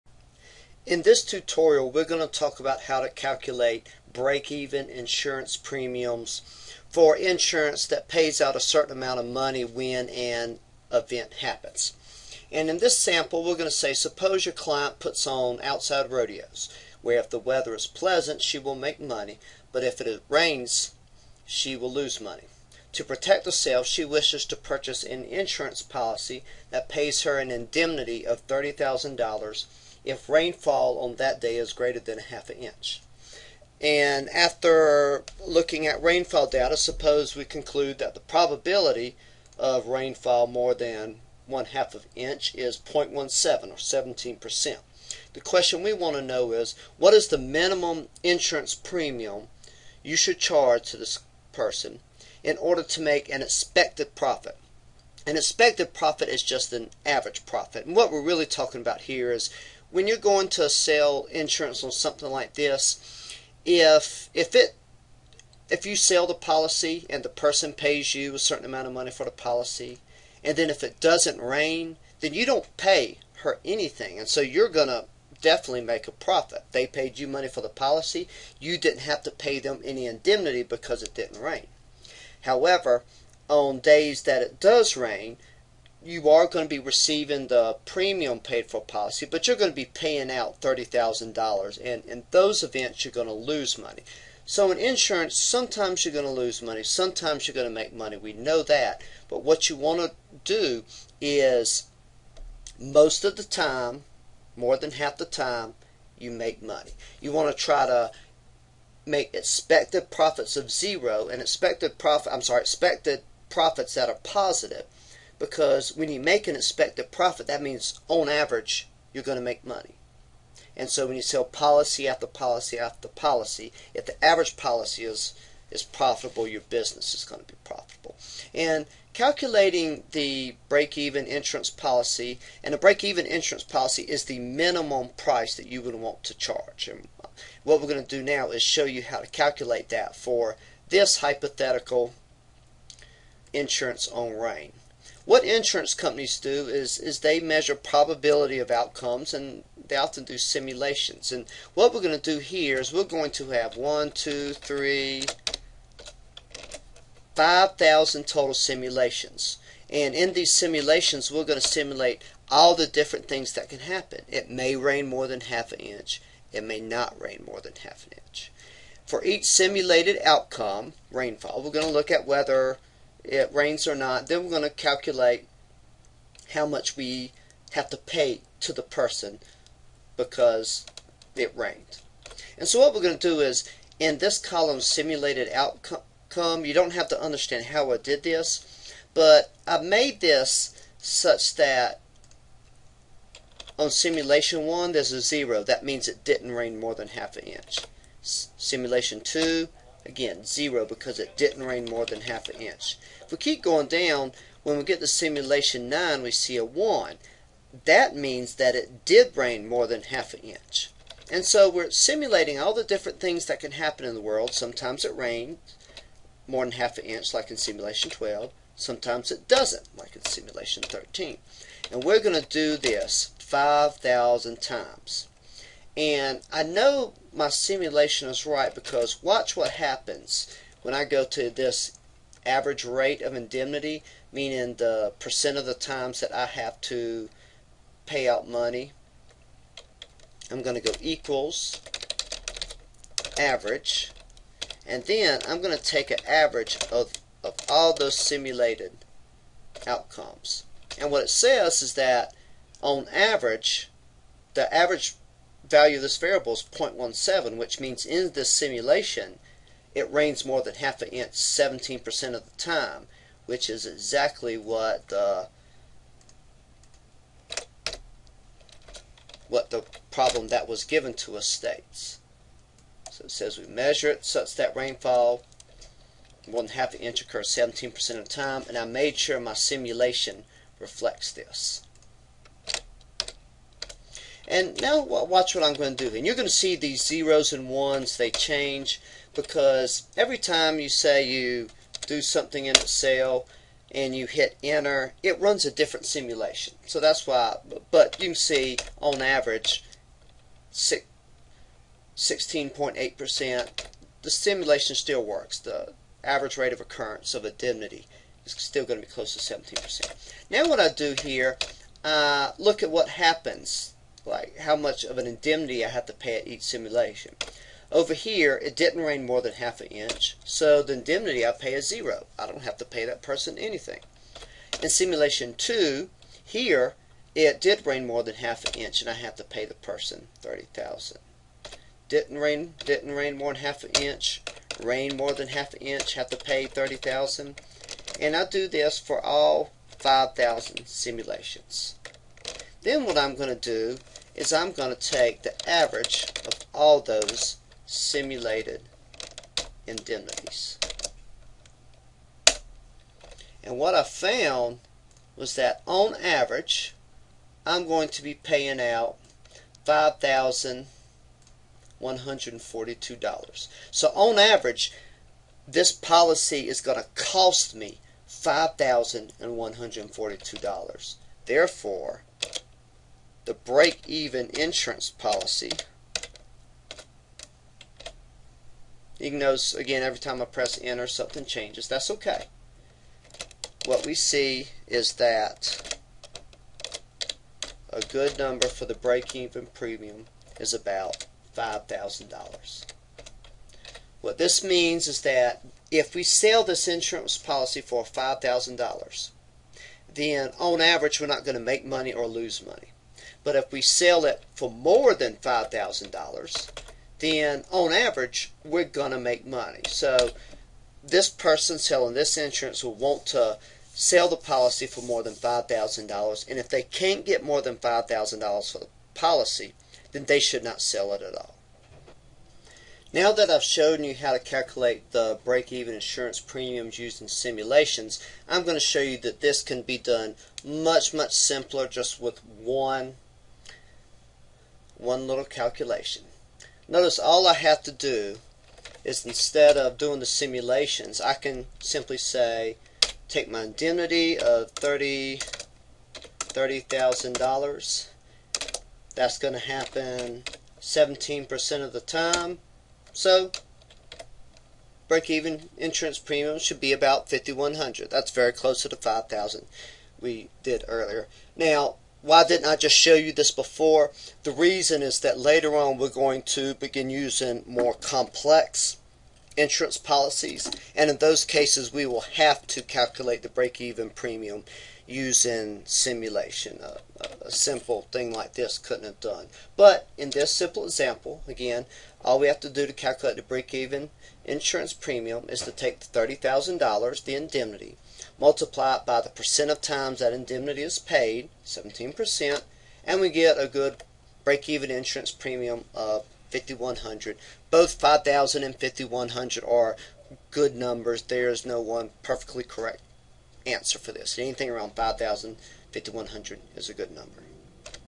Breakeven Insurance Premia
This tutorial demonstrates how to calculate breakeven insurance premia for an insurance policy that pays an indemnity of X, Y percent of the time.  It uses simulations (in additional to one multiplication) to demonstrate how premiums are calculated, and is a useful introduction to more advanced insurance policies.

Advanced Quantitative Methods in Agricultural Economics (AGEC 4213)
F. Bailey Norwood
Associate Professor
Department of Agricultural Economics
Oklahoma State University